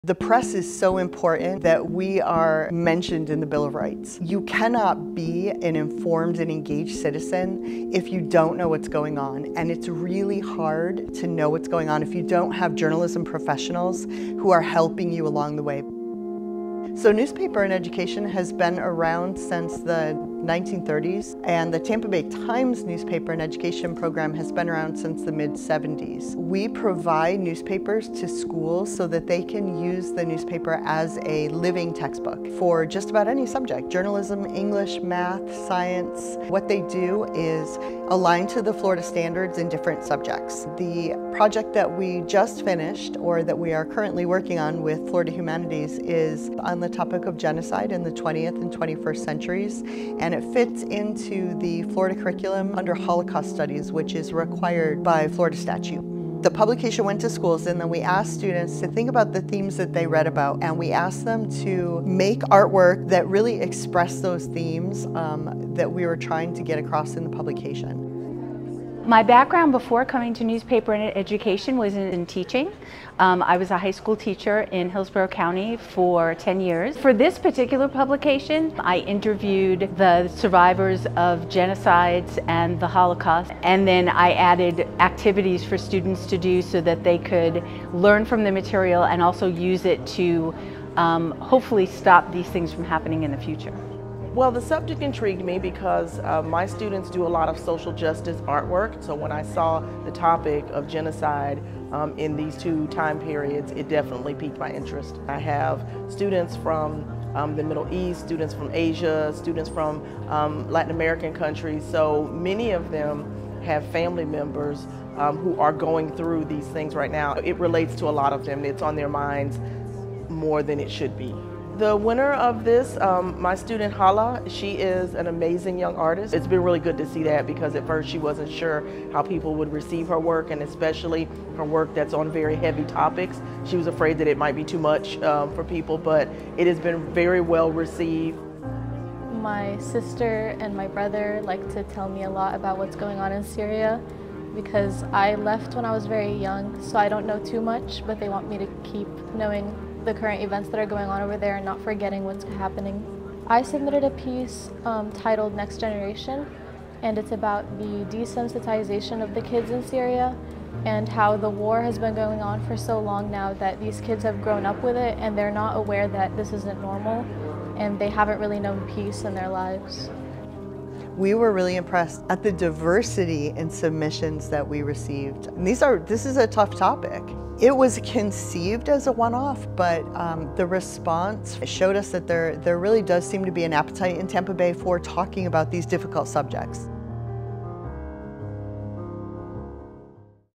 Funded Partner Spotlight | Tampa Bay Times Newspaper in Education
For roughly 50 years, the Tampa Bay Times Newspaper in Education Program has provided classrooms with educational publications and resources featuring a variety of subjects. In the last decade, Florida Humanities has supported five of these publications, ranging from the Civil Rights Movement to Democracy.

The most recent issue from NEI was funded by a Florida Humanities Community Project Grant and covered the topic of genocide in the 20th and 21st Centuries. What set this publication apart from previous ones was that Tampa Bay high school students were asked to create artwork on what they learned from the topic, which culminated in an art exhibition at @thefhm. Brooks DeBartolo Collegiate High School student Hala Albaba's piece, "A War-Raised Generation," won the art competition.